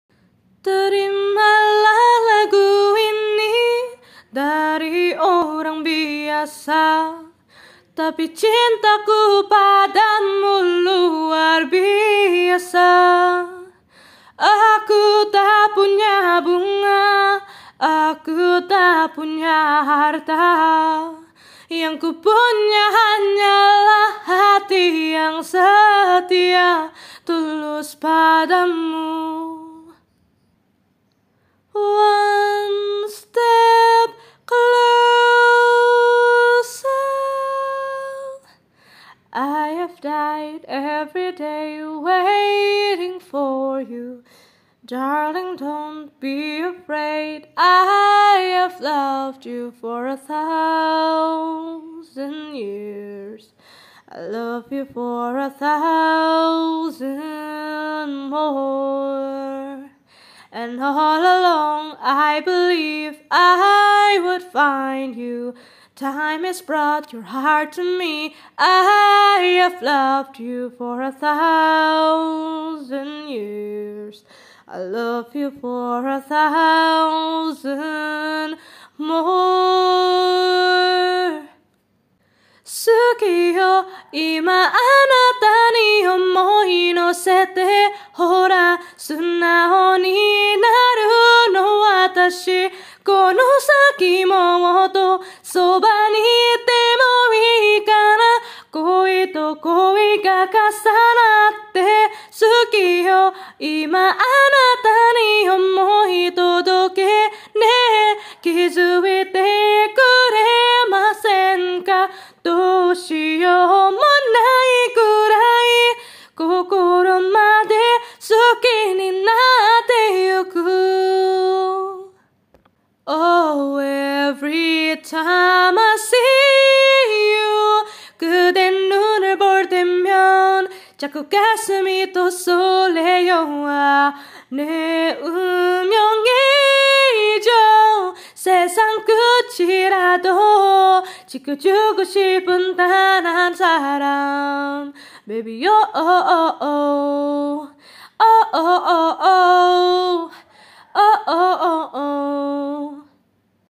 Songs that i think have similar vibes (IND, ENG, JPN, KOR)
I sing with my own voice
Acapella because I'm afraid i would get copyrighted
Songs i used are mentioned on the video screen